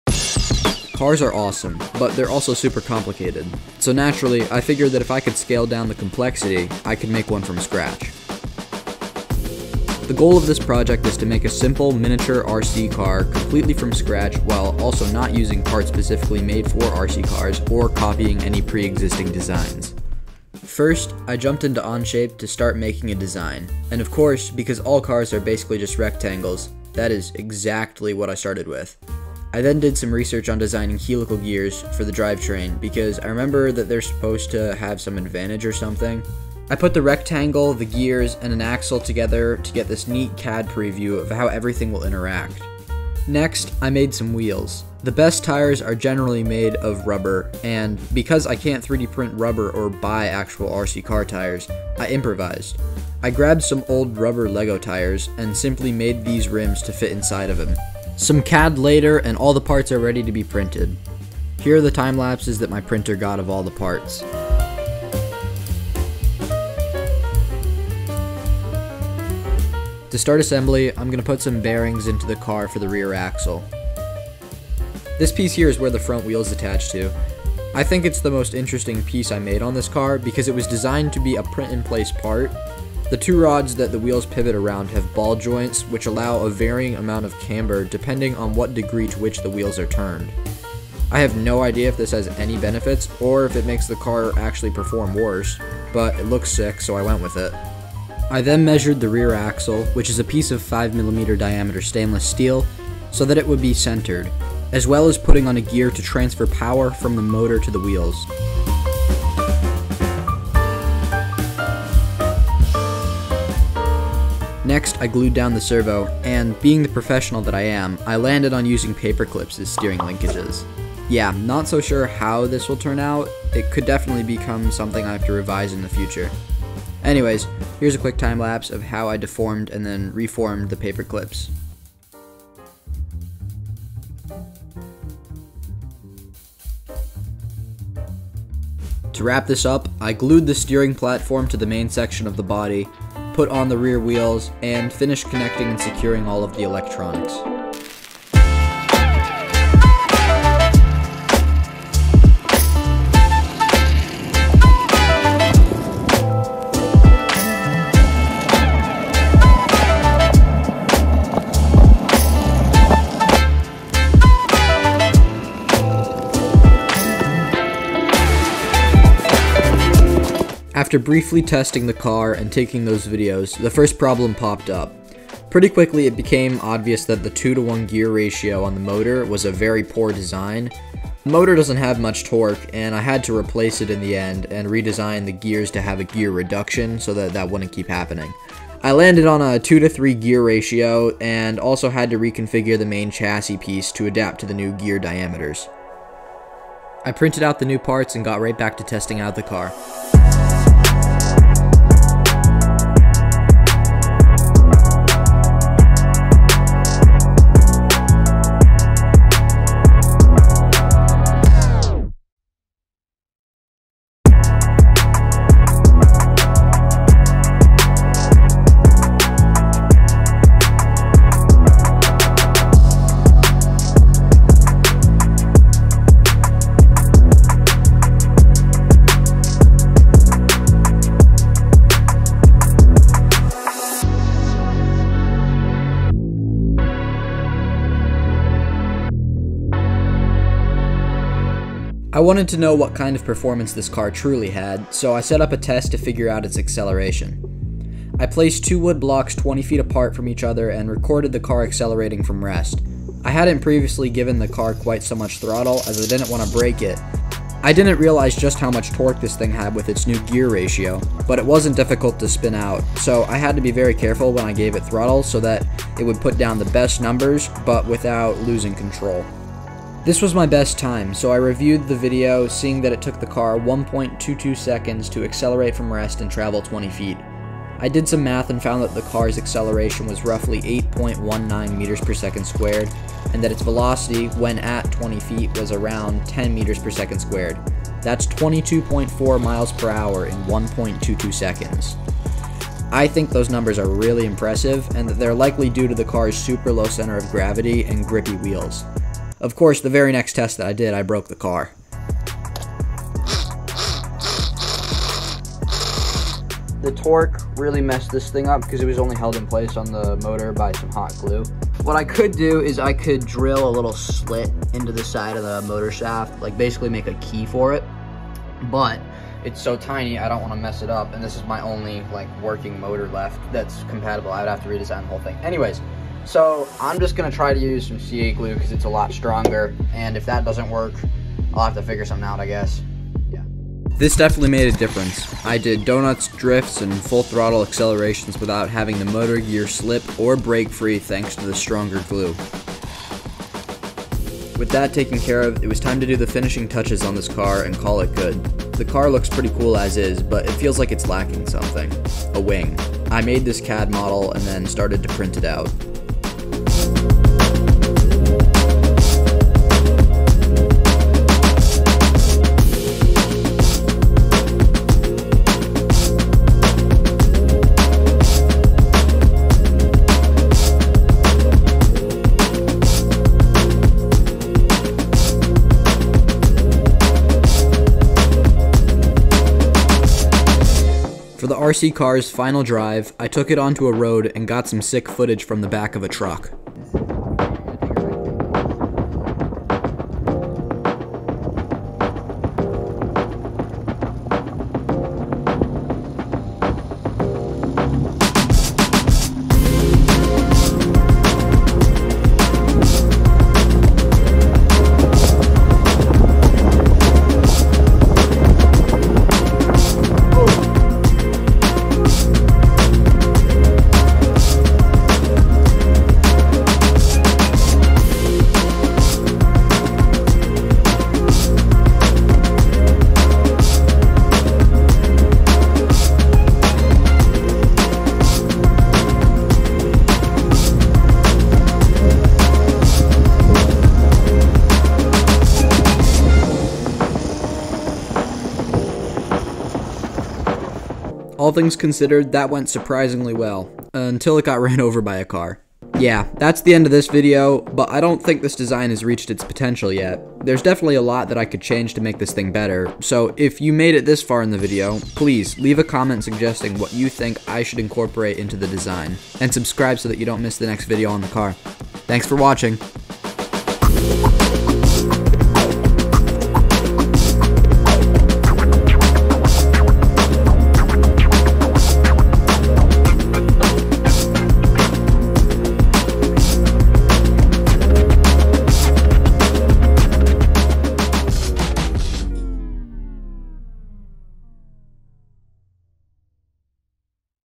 making a mini 3D printed RC car | 25mph
I had this idea randomly come to me, so I started this project which ended up taking shy of a month. I'll also put the CAD files up on thingiverse for those who are interested in recreating this thing.